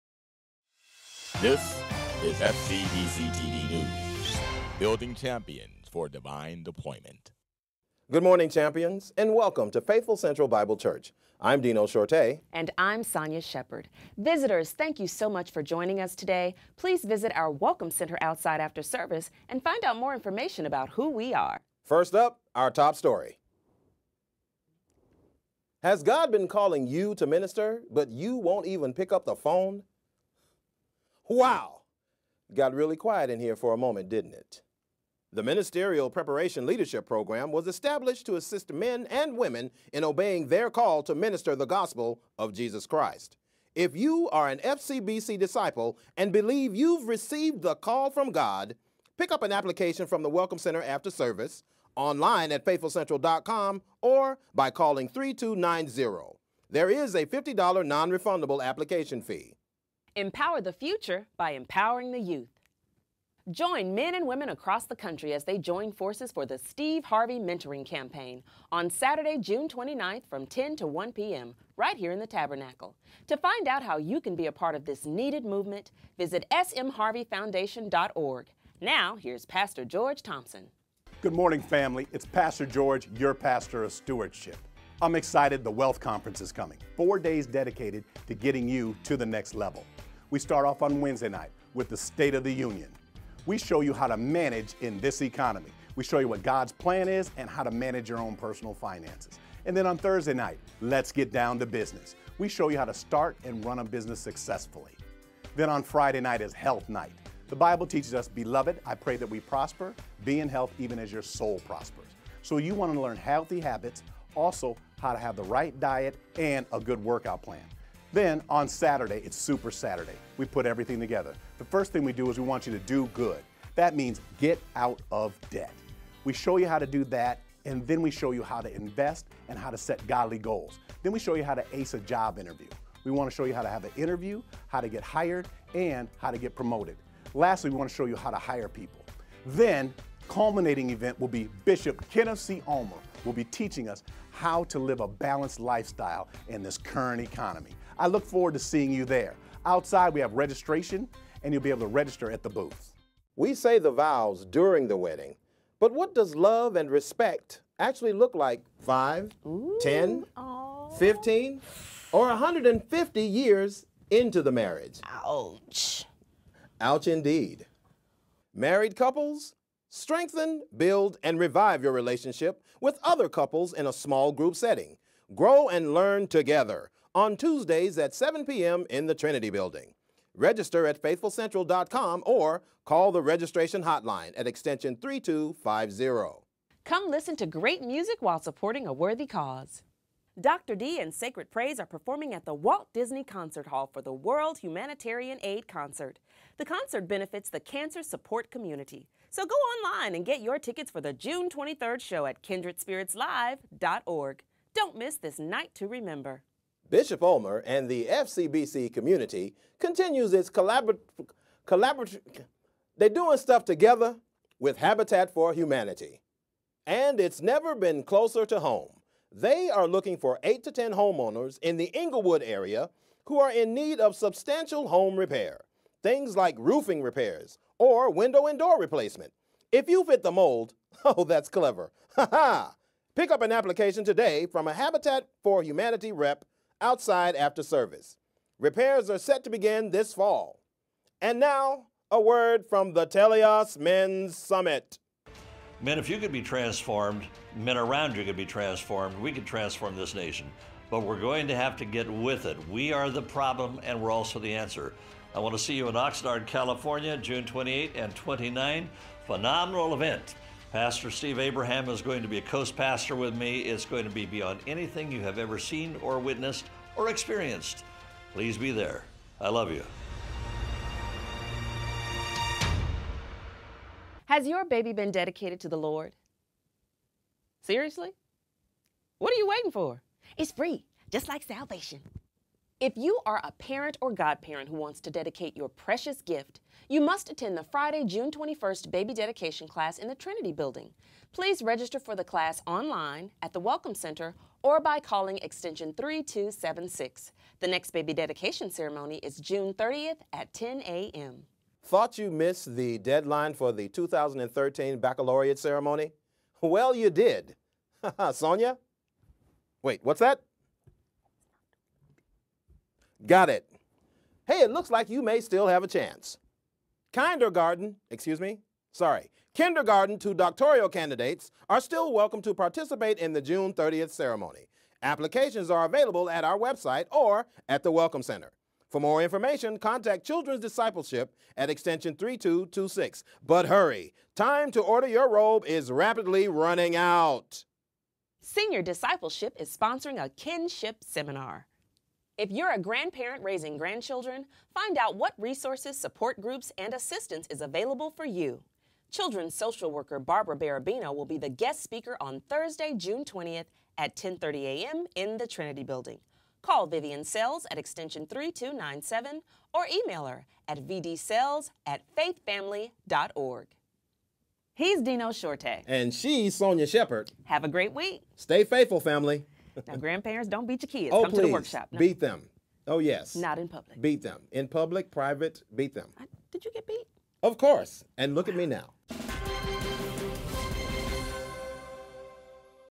FCBCtv News, June 9, 2013
Faithful Central Bible Church presents
FCBCtv News with Dino Shorte' & Sonya Shepherd

"Announcer" Guy: Carl Gilliard

Produced By The FCBC Production Services Department & The FCBC Repertory Company

Camera By: Peter Fenty

Sound & Edited By Peter Fenty

Produced, Written & Directed By Thomas Lazare